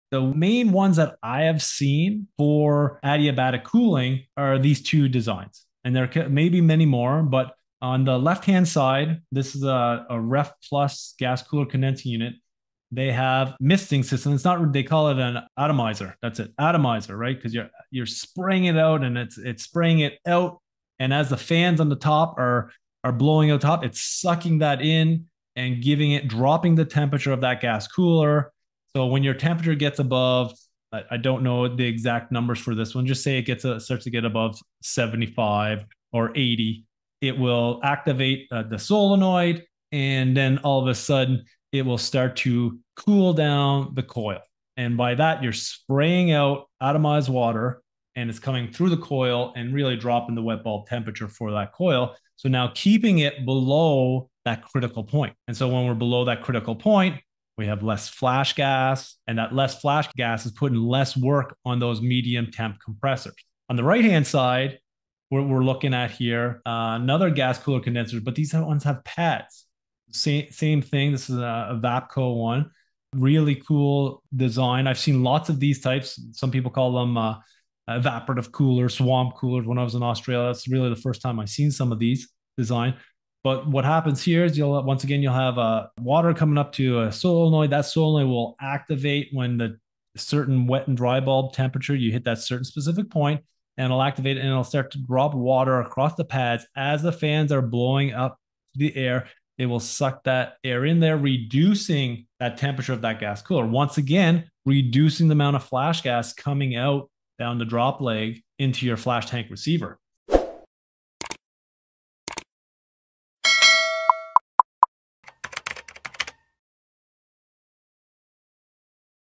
How does adiabatic cooling reduce flash gas in a CO₂ gas cooler?
This short explains two common adiabatic cooling designs used on gas cooler condenser units. Both methods focus on lowering air temperature entering the coil to improve system performance during higher ambient conditions.

• Atomized water misting systems activated by temperature conditions
• Wet-bulb temperature reduction across the gas cooler coil
• Pad-based evaporative designs that cool incoming air
• Reduced flash gas entering the flash tank
• Lower compressor workload and improved efficiency

Understanding these designs helps technicians recognize how CO₂ systems manage heat during warm weather operation.